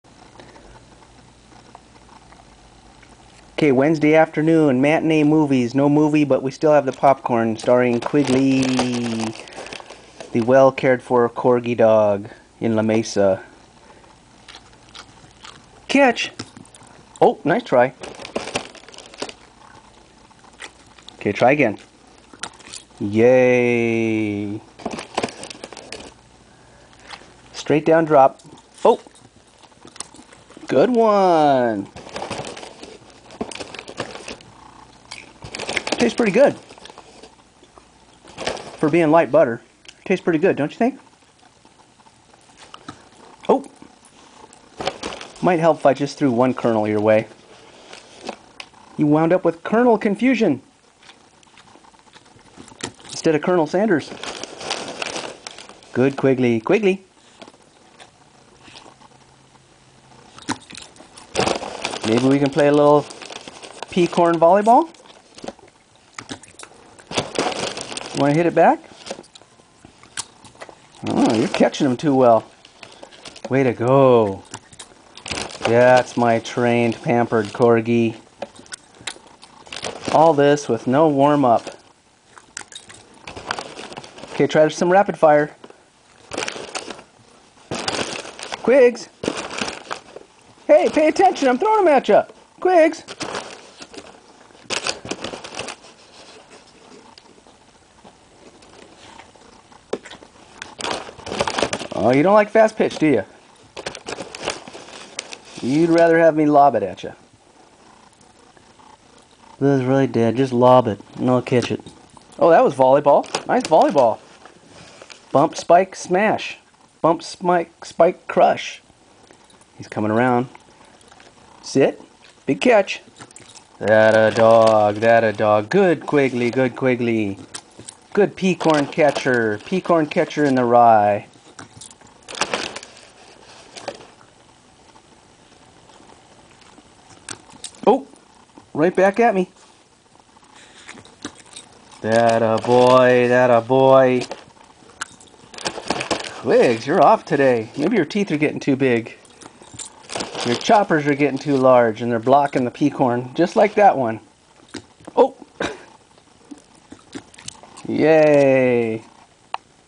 Micro-Pop Lovin' Welsh Corgi dog, Uncensored!!!
Our 'works from home' Welsh Corgi dog shows his prowess in Popcorn catchery, on a nondescript mid week afternoon.  Sequel to be in 3-D.
Hey, Can ur dog do this...?